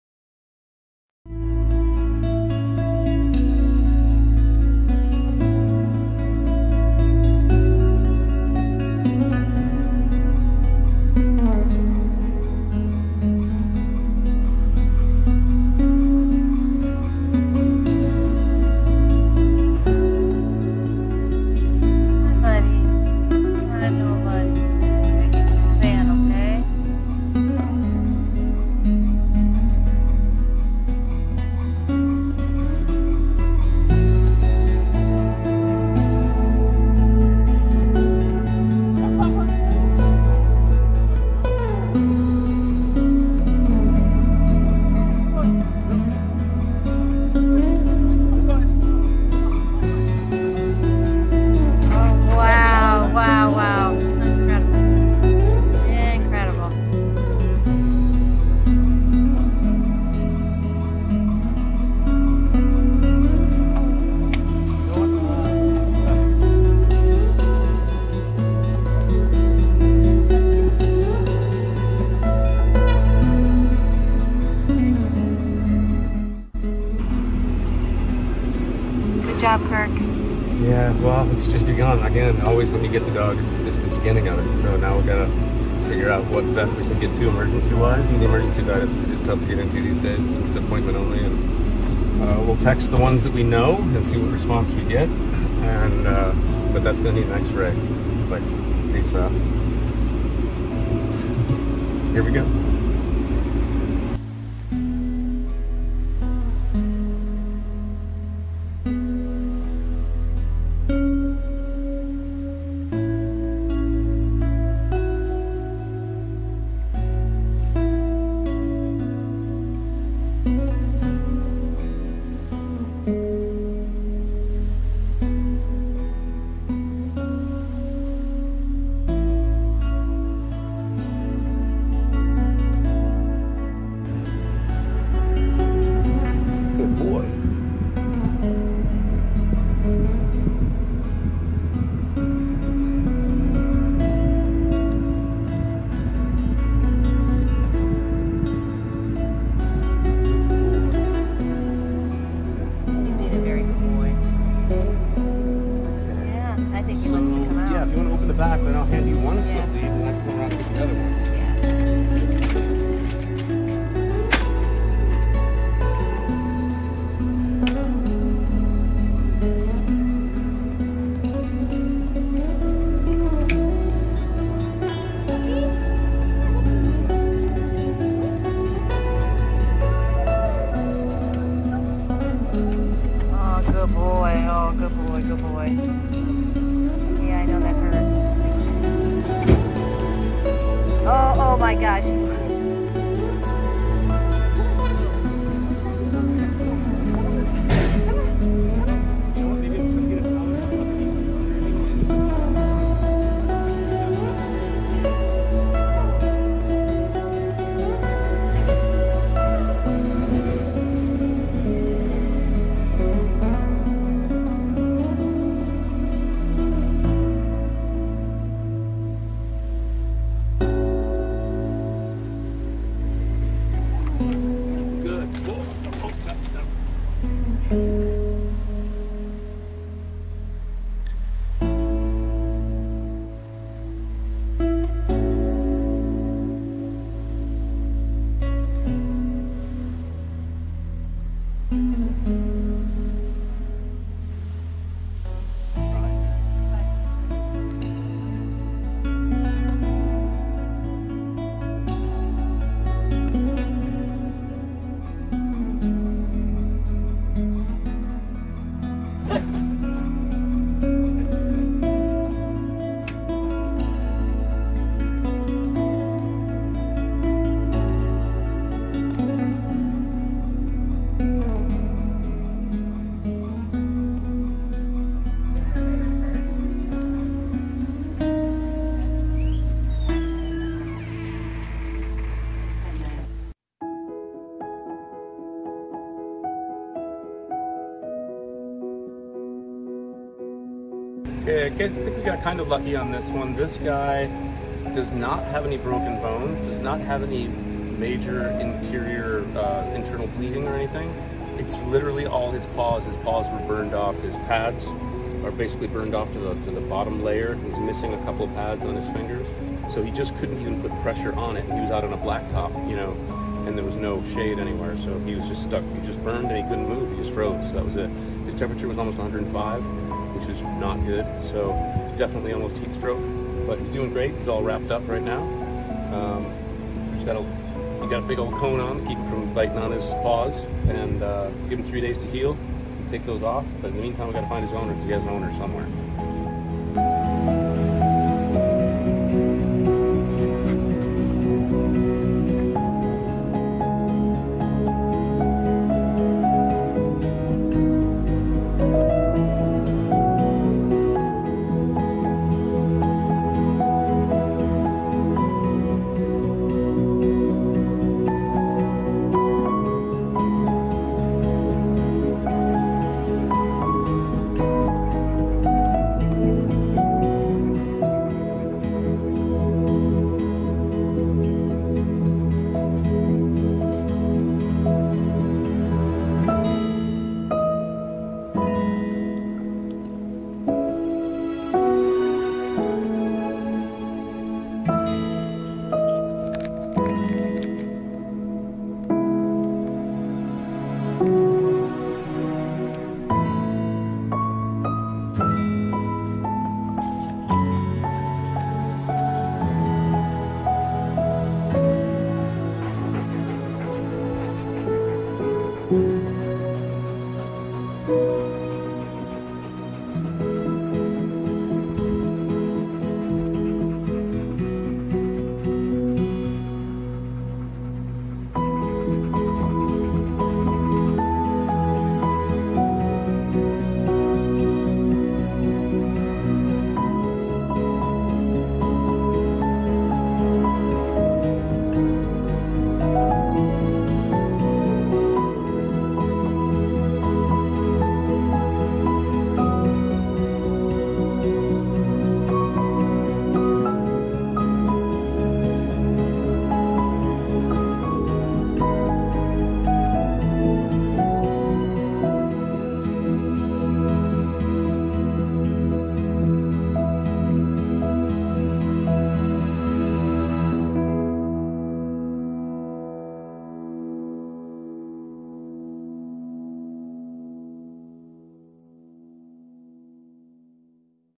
Belgian Malinois Rescued From 114 Degree Heat
We got a call about a dog who was lying in the middle of the road in Thermal for over an hour and could not move. They were not sure if he had been hit by a car but there was blood on his paws. It was 115 degrees and the dog was sitting on hot pavement. The heat alone was going to kill him. His situation was dire.